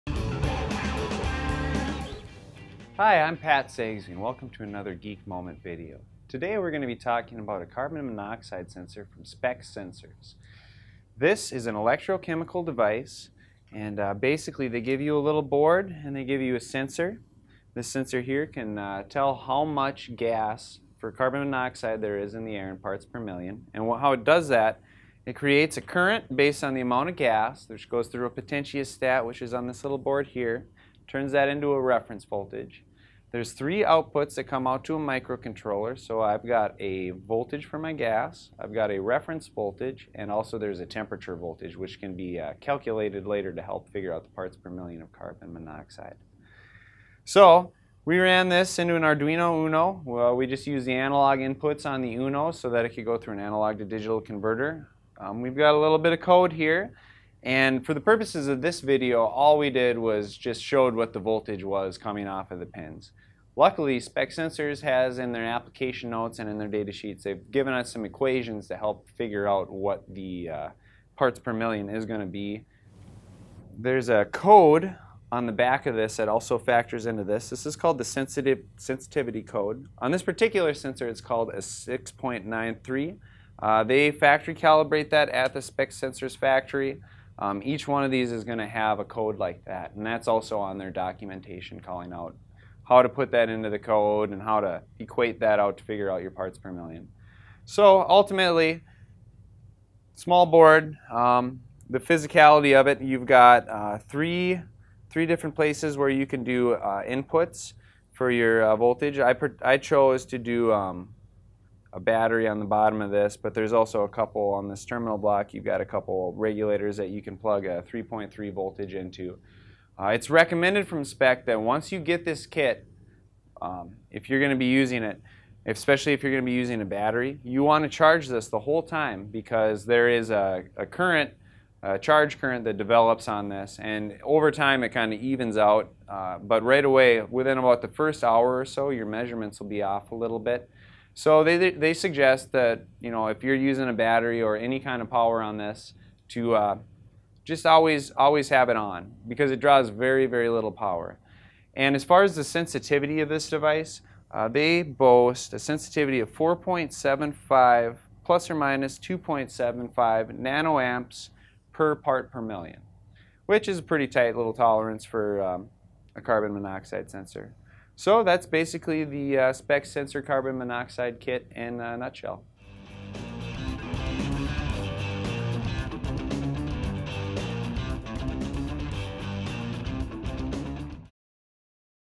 SPEC Sensors Carbon Monoxide Sensor Kit – Another Geek Moment | DigiKey
This time on Another Geek Moment, Pat from Digi-Key shows us how to set up the SPEC Sensors Carbon Monoxide Sensor Kit.

The 968-020 Carbon Monoxide Sensor Kit from SPEC Sensors provides you with the sensor module (ULPSM) that is configured to work specifically with Carbon Monoxide gas, an evaluation board that the ULPSM can directly plug into, two additional sensors to work with the ULSPM and 2 sensors for use in your PCB design.  Pat is using an Arduino Uno to interface with the evaluation kit.  The full project documentation can be found on the eeWiki.